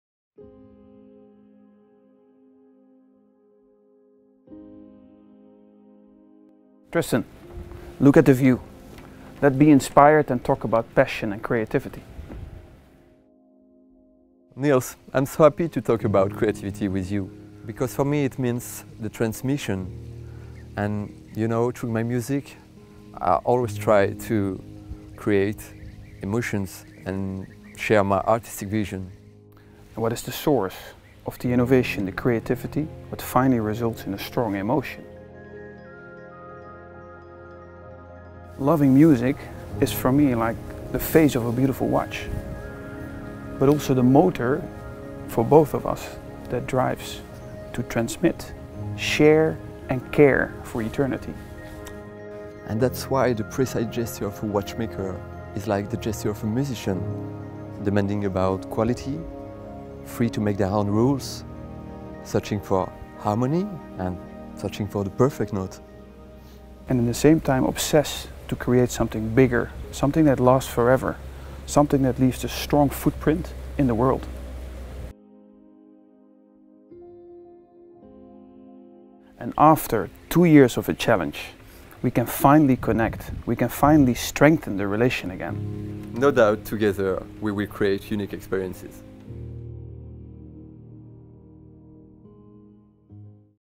Frederique Constant Timeless Walks - The Avener, Highlife Ambassador
Frederique Constant is proud to announce its collaboration with the French musician and producer The Avener, as ambassador of its Highlife collection. Timepieces embodying innovation, creativity and modernity, all features that the artist injects into his music. Producing his own compositions, he also reprises classics in an original electropop style, punctuated with notes of blues, jazz, folk or soul. Driven by a burning passion, musicians and watchmakers share the same quest for excellence and precision aimed at producing moments of exception through their art.